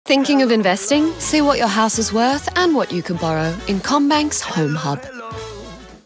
Thinking of investing? Discover your property’s potential through Home Hub, in the CommBank app.
Wondering if your property has generated any equity? Did you know you can see if you have any usable equity and more in Home Hub on the CommBank app.